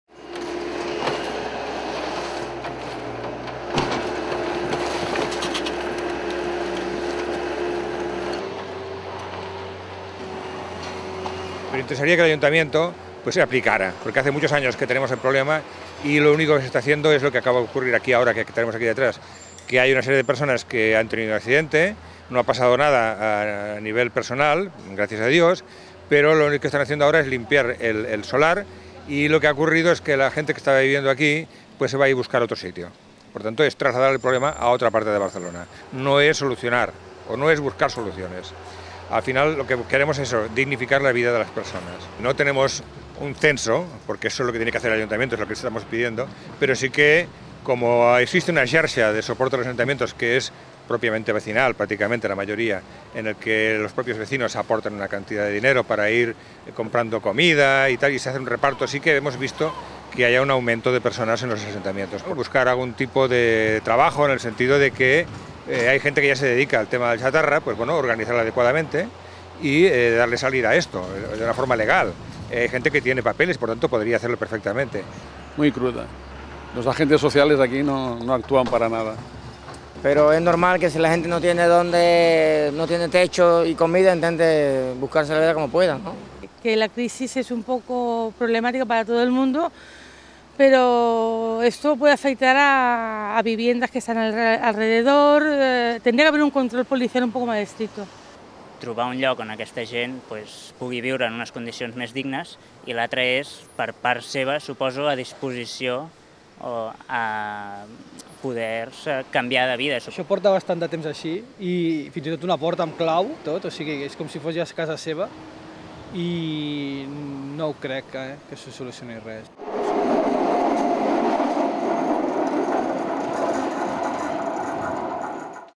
Aflora el chabolismo en el distrito tecnológico de Barcelona
Los incendios sufridos en dos asentamientos en dos meses crean inseguridad entre los vecinos - Jesús Sancho, Ana Jiménez